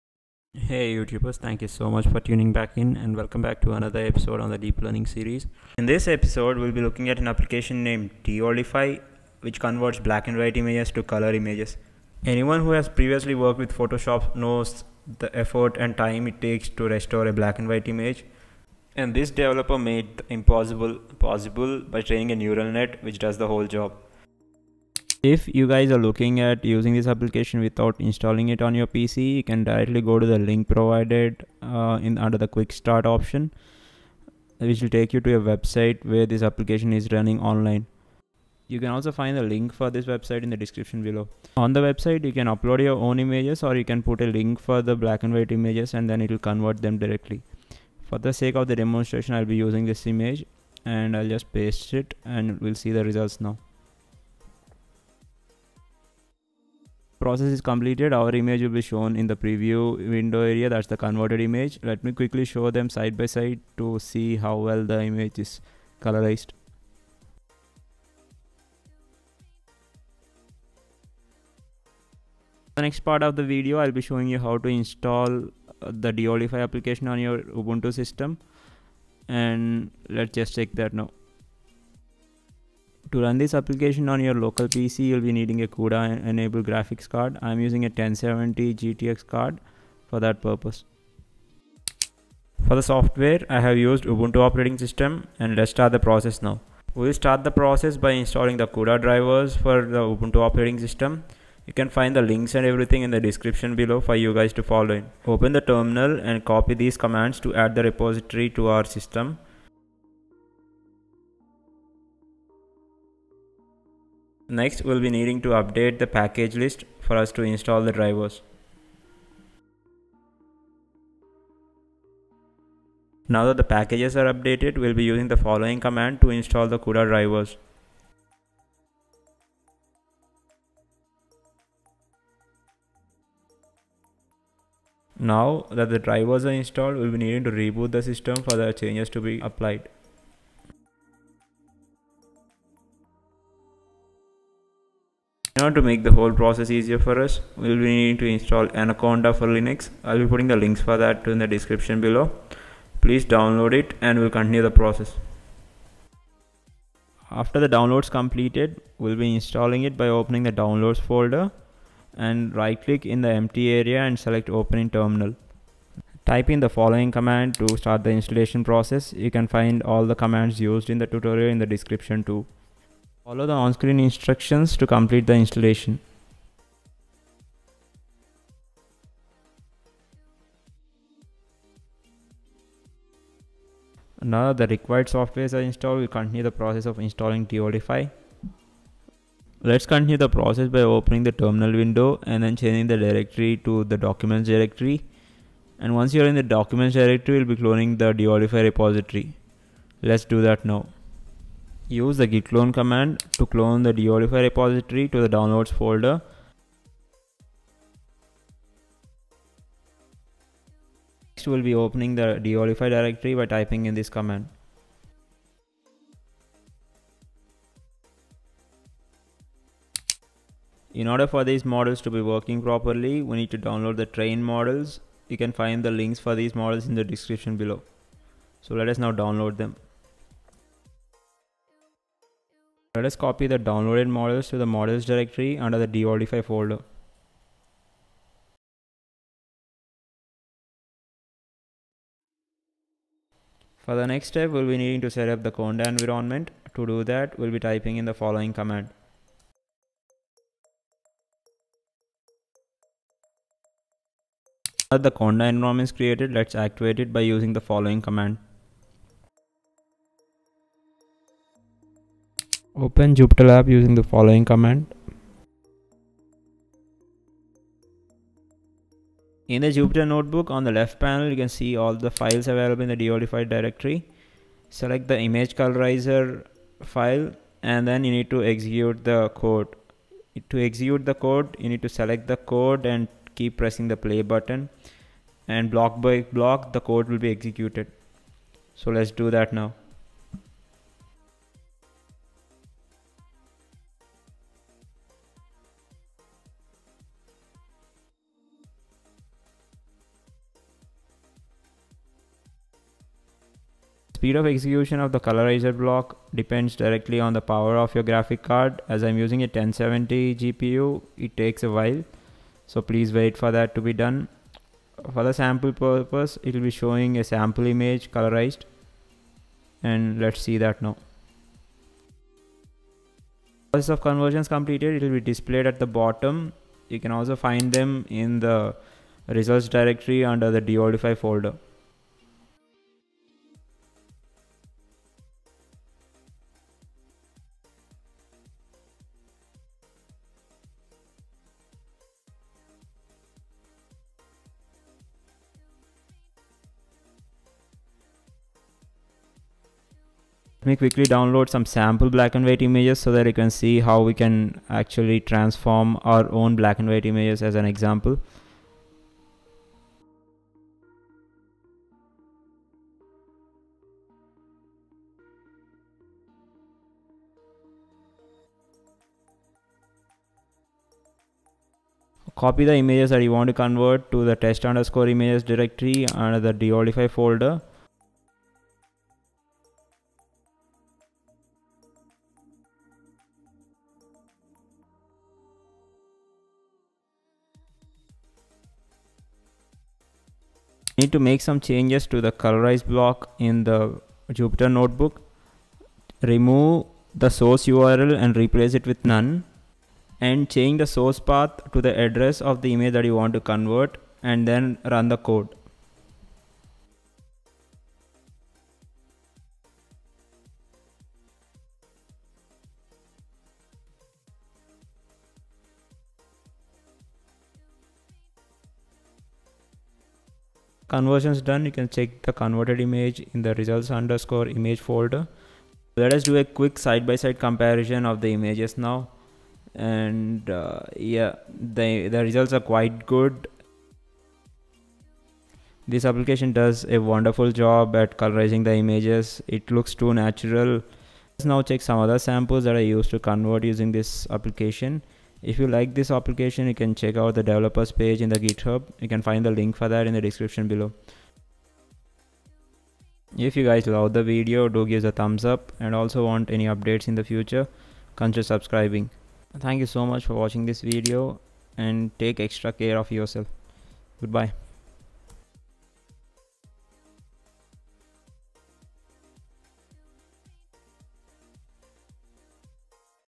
DeepLearning series Ep 7 : DeOldify AI based Image Coloriser
DeOldify

Hardware Required
Cuda enabled Nvidia Cards

Hardware and OS used
Ububntu 18.04 and Msi gtx 1070

Install Nvidia Drivers with CUDA support
1.Select a driver repository for the CUDA Toolkit and add it.
2.Update the package lists.
sudo apt-get update
3.Install CUDA, which includes the NVIDIA driver.
sudo apt-get install cuda
source:

reboot your system

Verifying the GPU driver install :
nvidia-smi

Download trained models
Stable Generator

go to cloned Deoldify folder using
cd DeOldify

conda env create -f environment.yml

conda activate deoldify

open jupyterlab using
jupyter lab